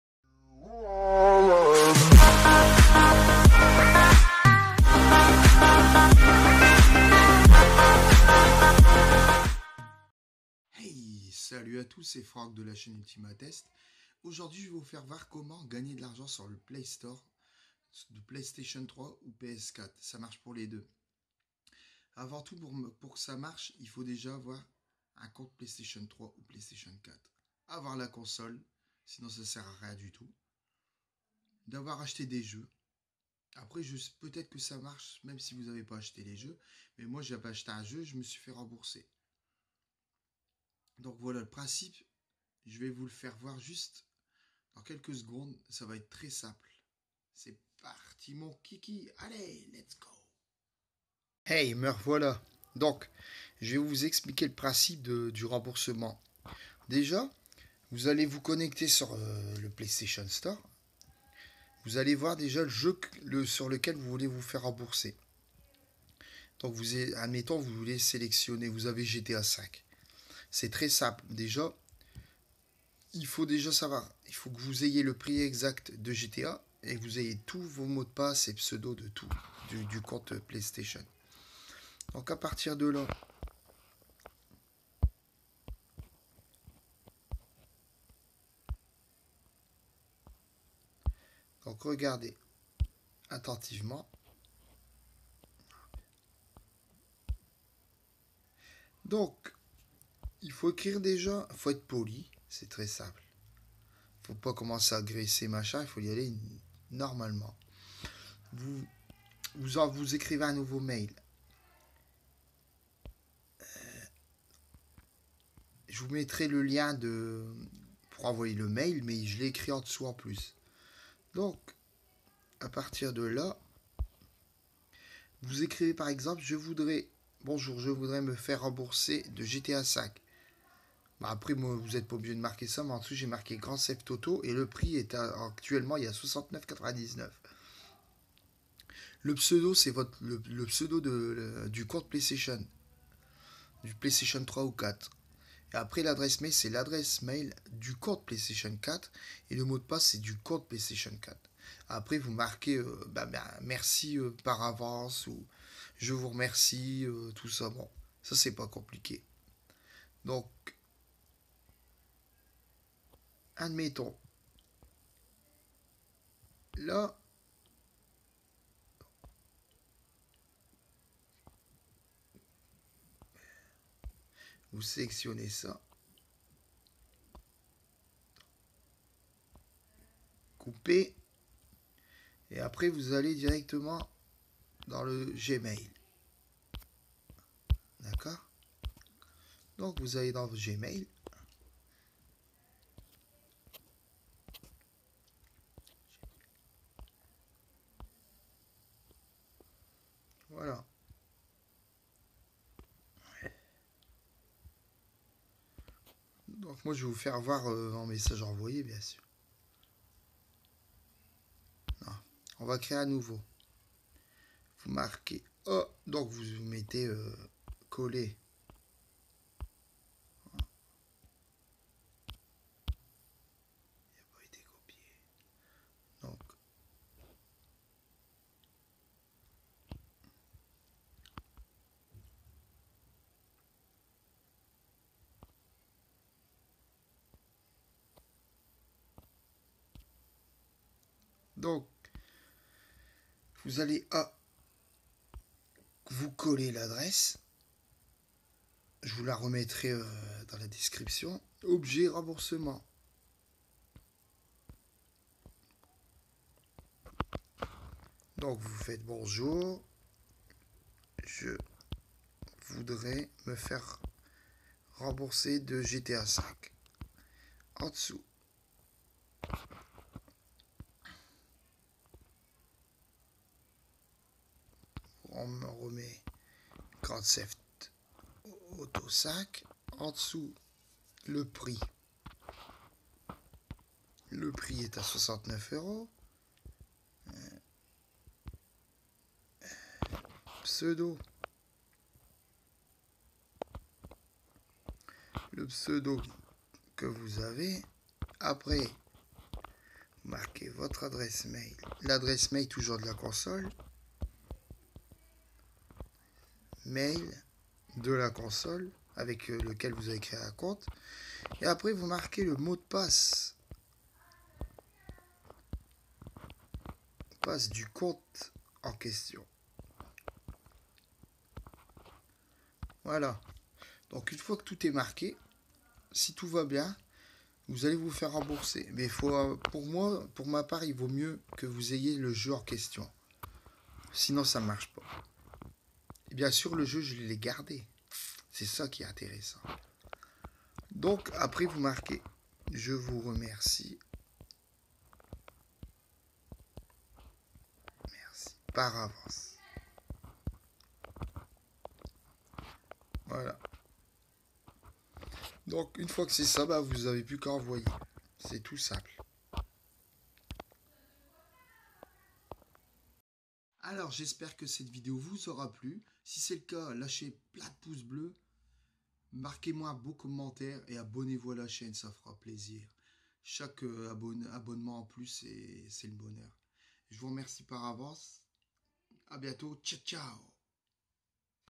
Pstore les jeux remboursées. Profitez-en
Je vais vous montrer comment avoir touts vos achats de jeux pstore remboursé.
Sa marche bien je me suis fait rembourser de deux jeux.
Profiter-en
Le lien de l'adresse mail.:

Abonné vous
Max de pouce bleu
Facebook UltimateTest
Snapchat UltimateTest
Instagram UltimateTest
Twitter UltimateTest
Merci